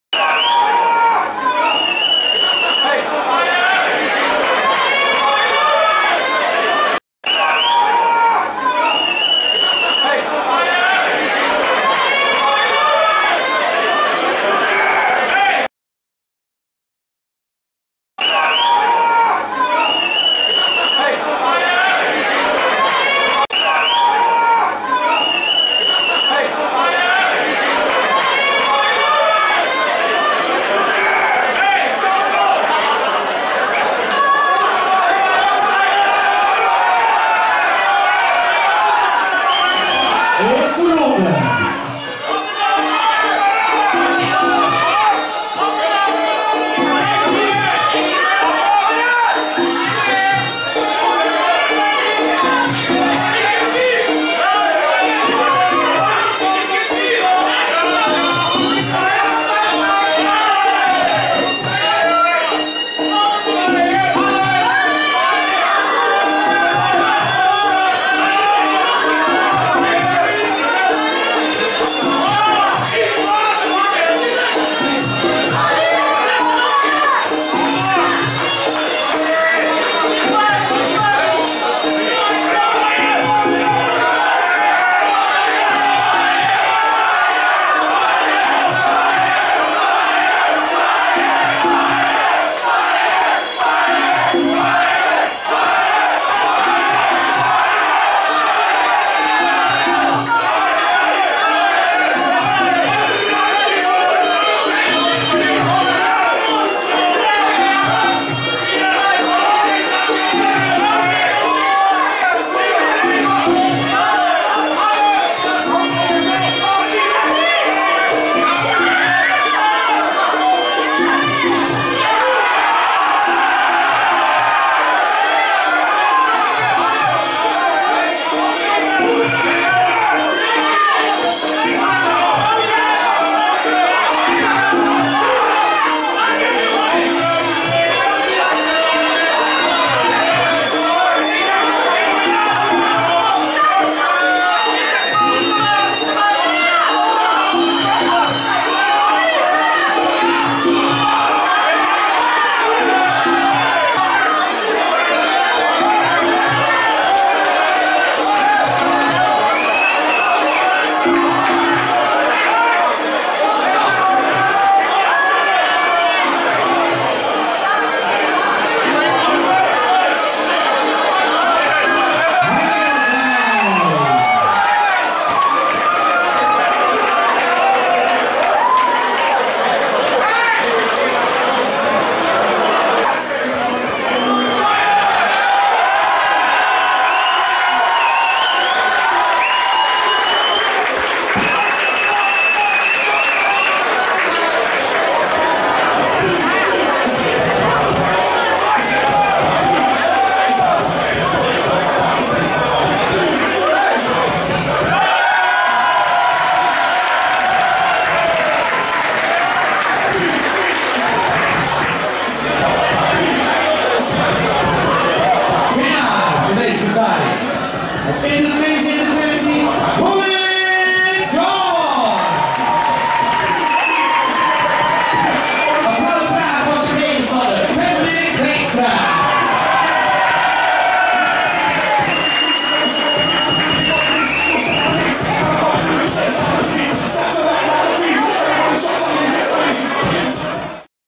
Patrick Veenstra (Peye) Colin George
A-Class  Patrick Veenstra (Elsomo Gym) vs Colin George (Mike's Gym) @ Muay Thai event Elsomo, Kruisweg Marum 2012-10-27